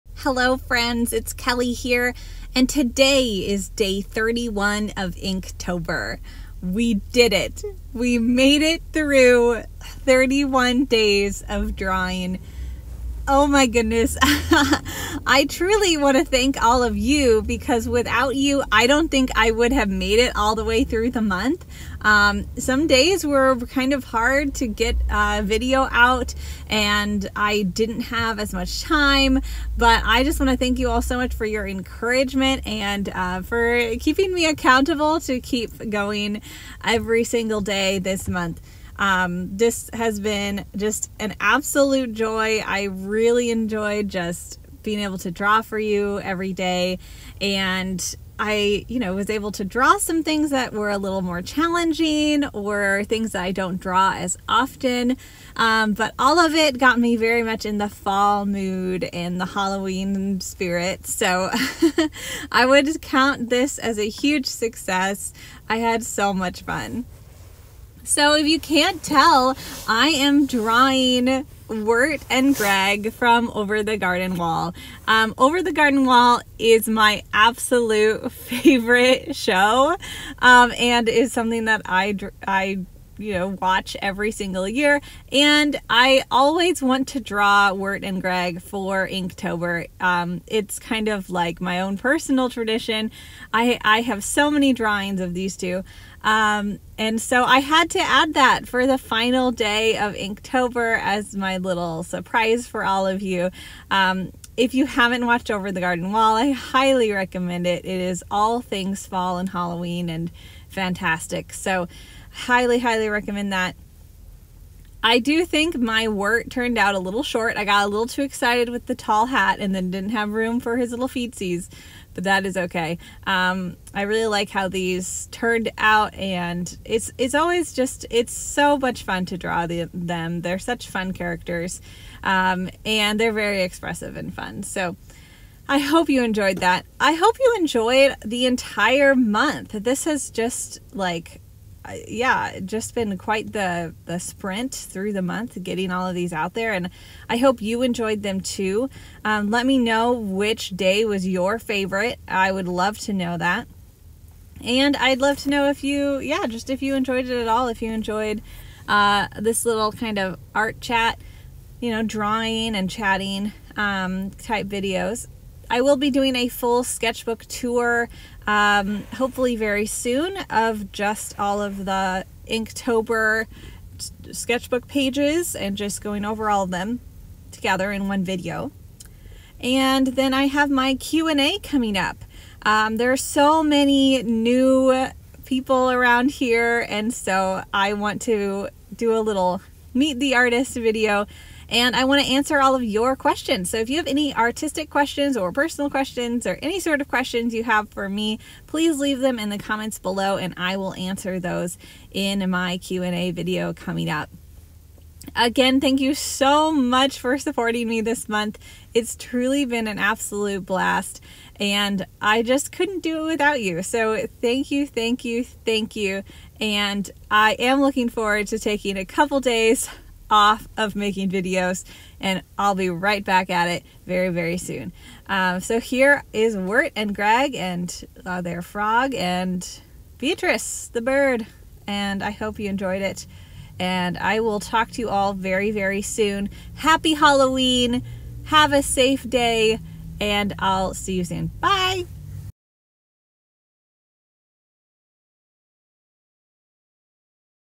over the garden wall | daily draw with me | Inktober 2023
Surprise ending with Over the Garden Wall! Thanks so much for supporting me through this month. I'm so proud we made it through all 31 days of drawing! I hope you enjoyed this Inktober, and I couldn't end the challenge without drawing the best fall boys out there. Happy Halloween!